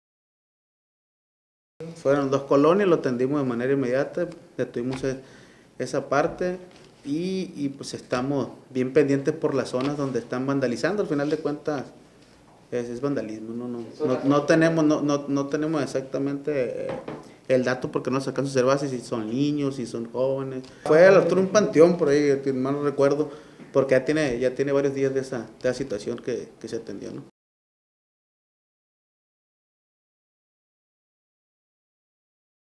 Policía Municipal ya investiga ataques a camiones urbanos en Las Mañanitas
Pasajeros reportaron ataques con piedras en algunas colonias